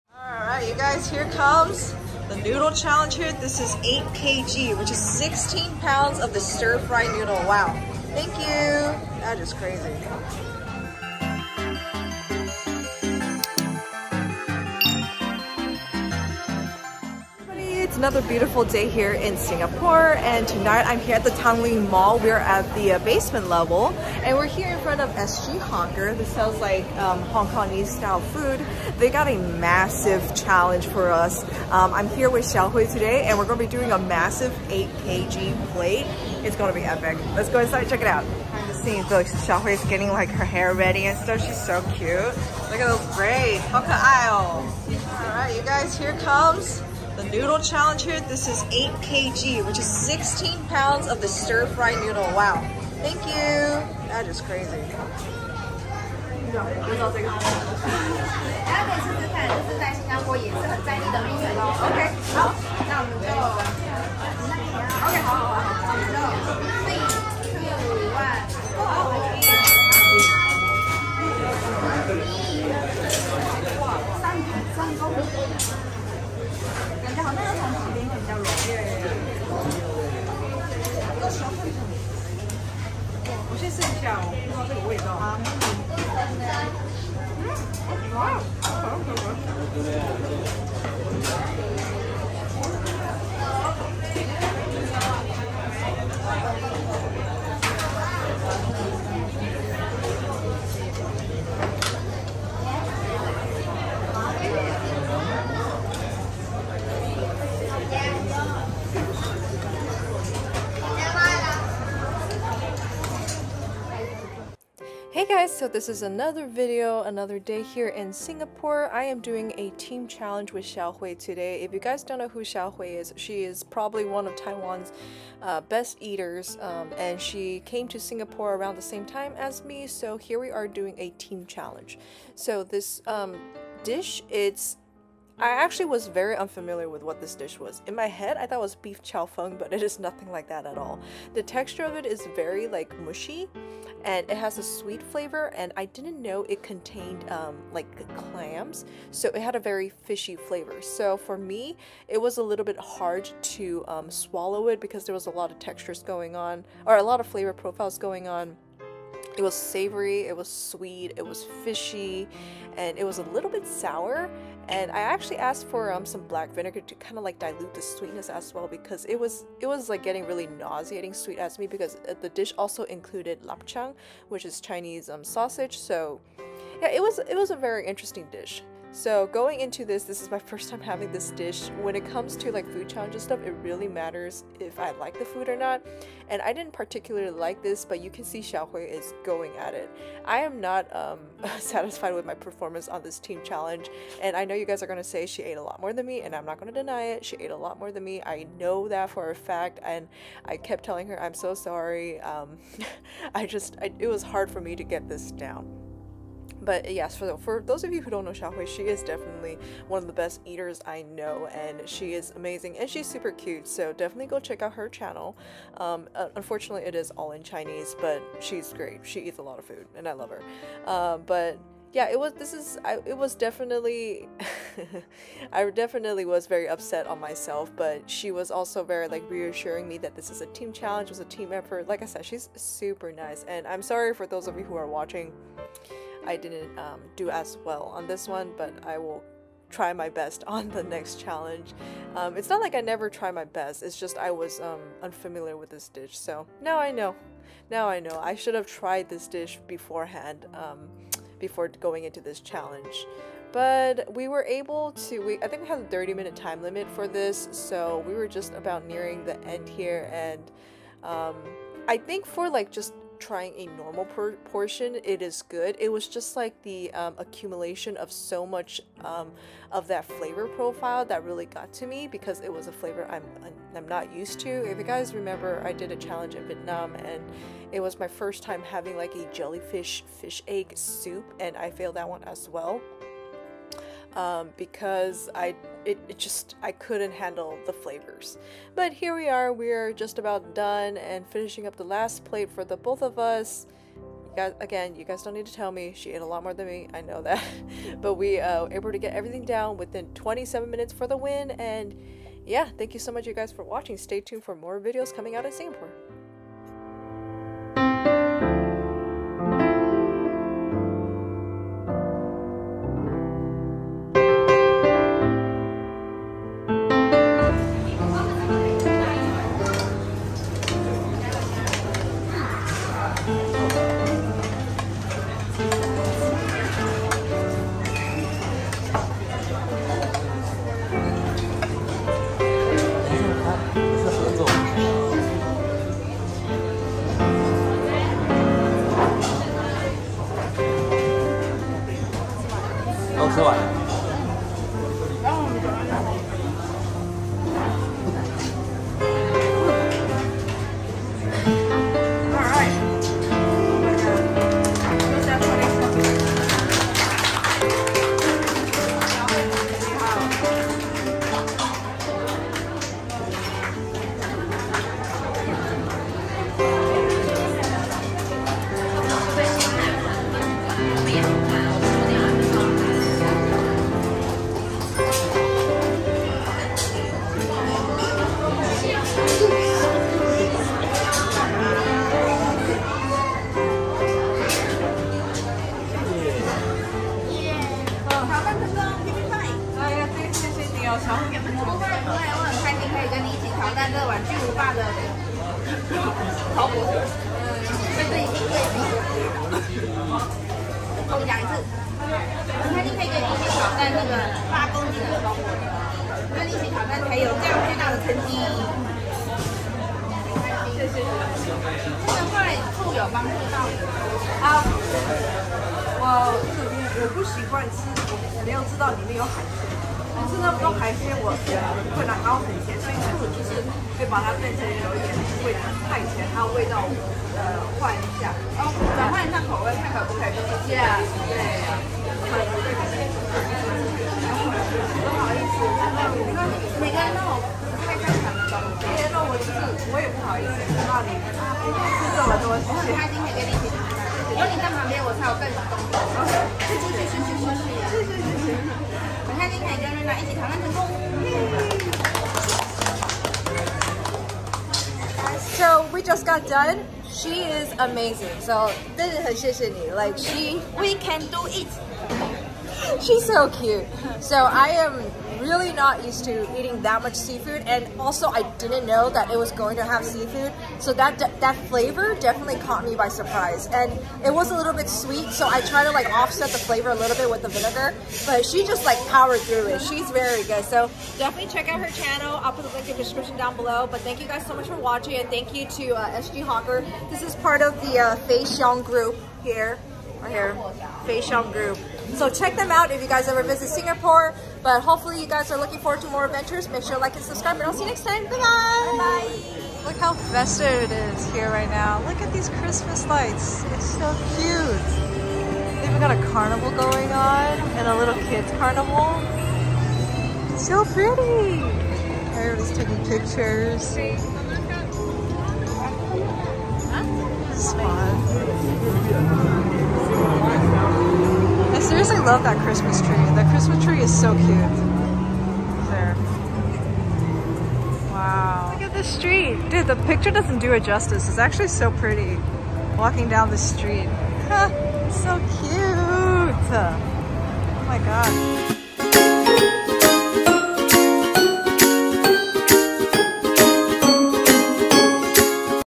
16LBS OF FOOD CHALLENGE FT. @xiaohui_foodie IN SINGAPORE!!
check out @xiaohui_foodie

20687 Amar Rd Ste 2-196

Here are all my media links